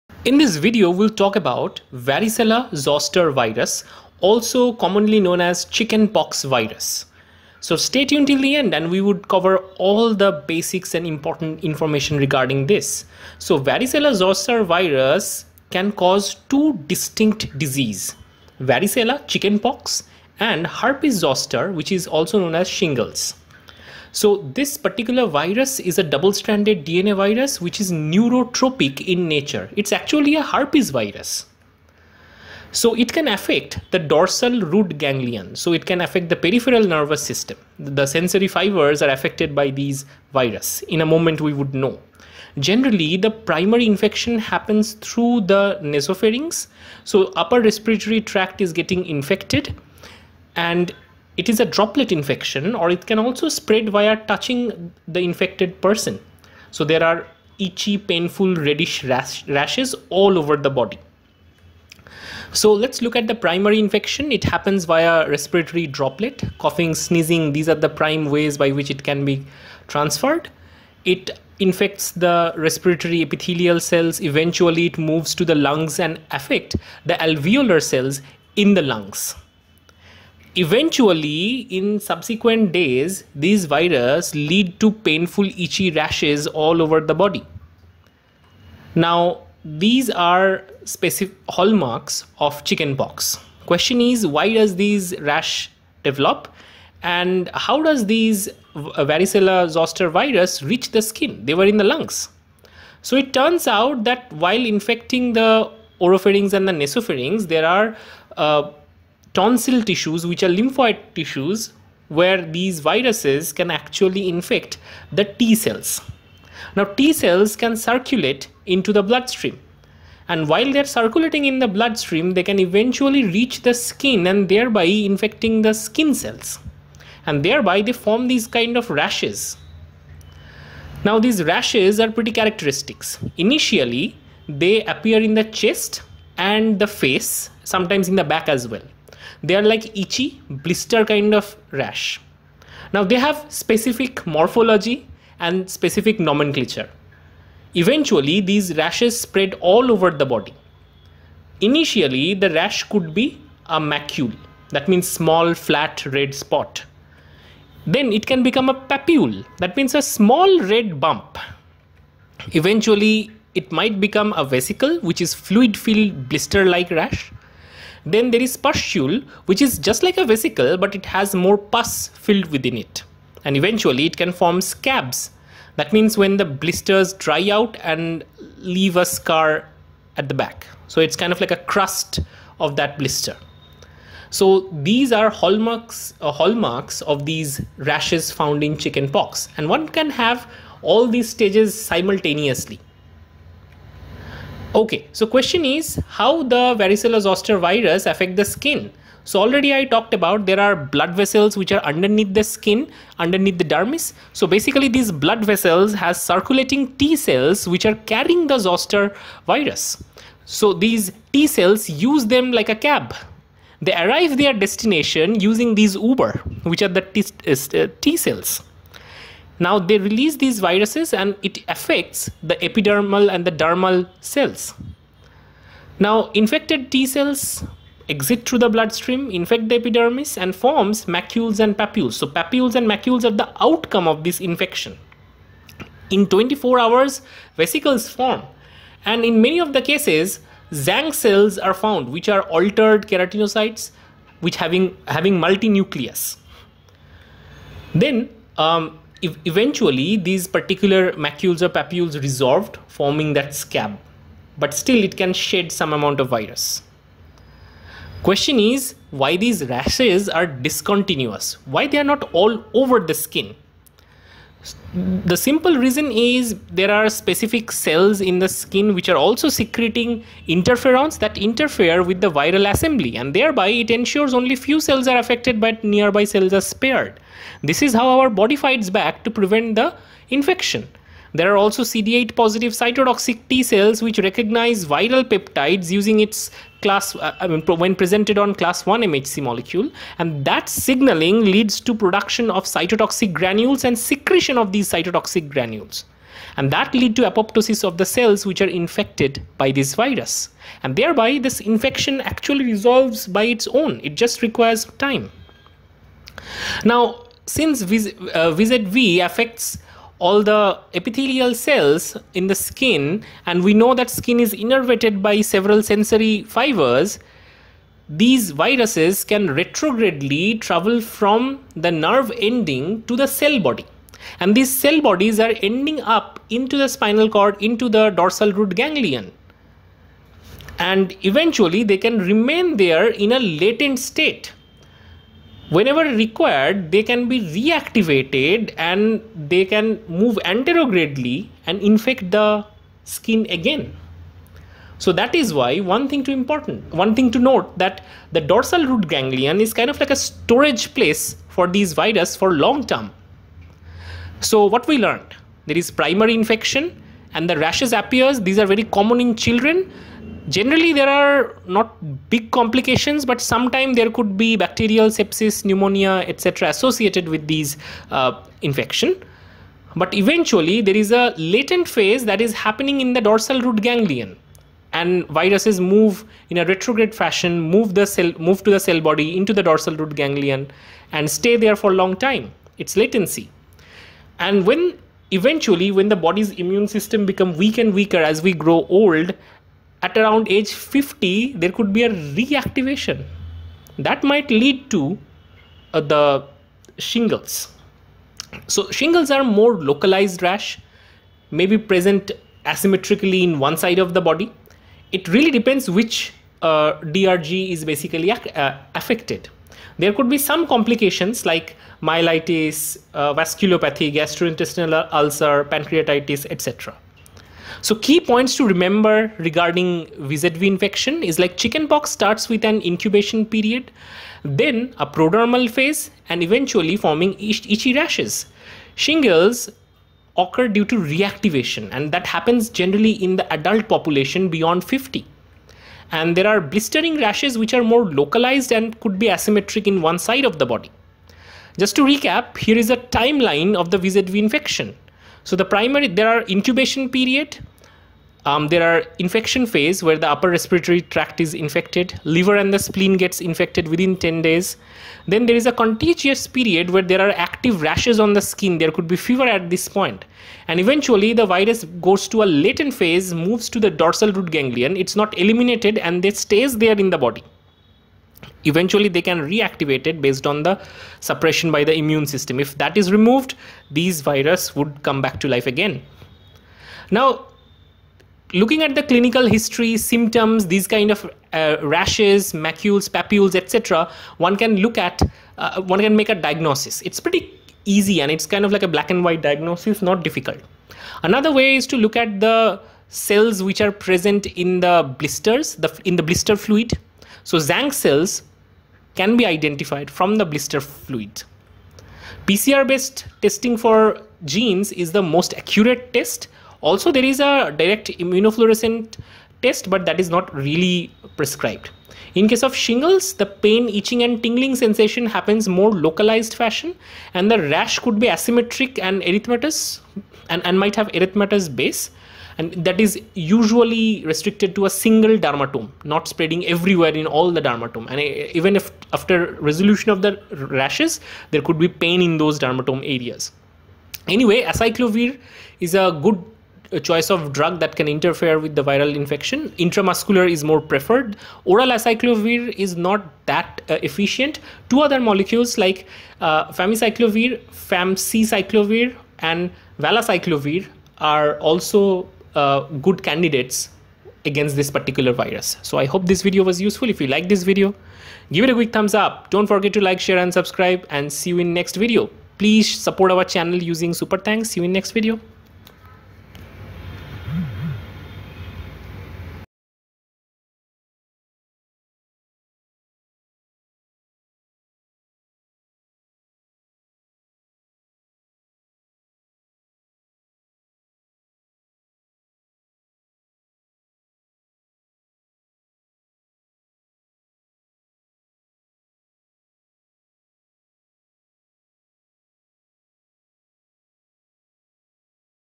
Varicella Zoster Virus | Chicken pox and Shingles | pathology , diagnosis and treatment
This video talks about Varicella Zoster Virus | Chicken pox and Shingles | pathology , diagnosis and treatment.